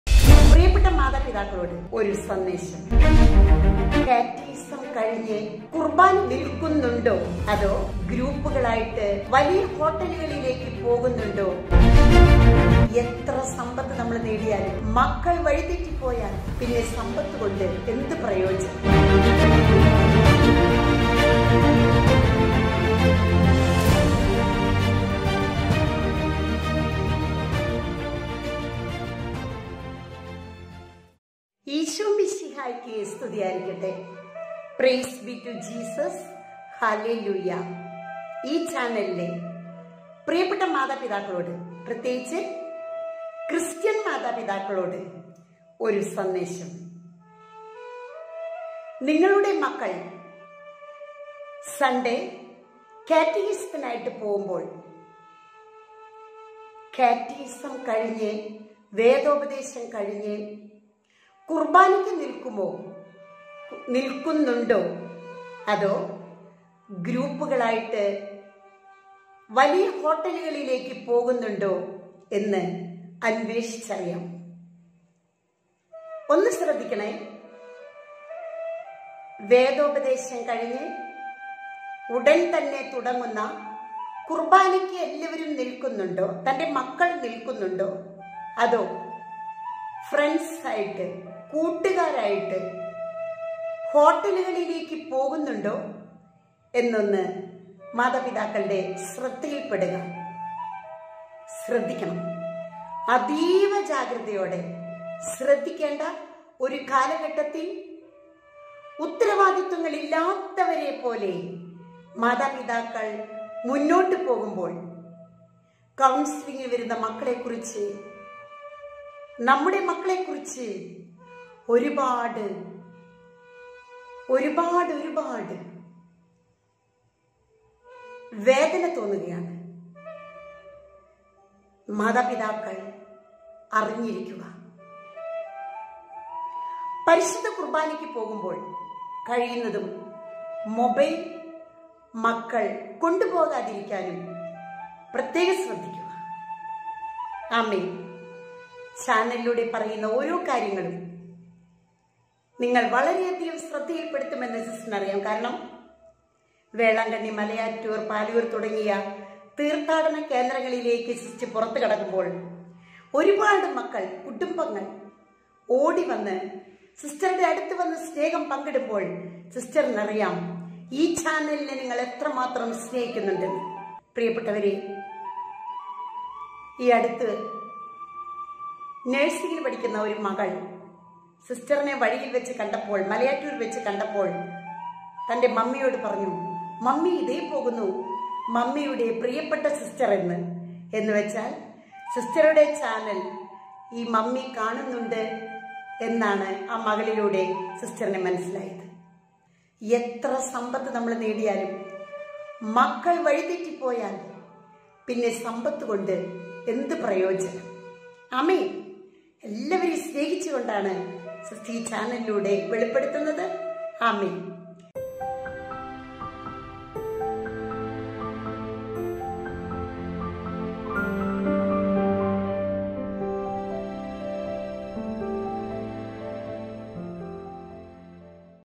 ഞായറാഴ്ച വേദപാഠം കഴിഞ്ഞ് നമ്മുടെ മക്കൾ പോകുന്നത് എങ്ങോട്ട്..?
കന്യാസ്ത്രീയുടെ ഞെട്ടിക്കുന്ന വെളിപ്പെടുത്തൽ..!

സി. ഡോ കാർമ്മൽ നീലങ്കാവിൽ  CMC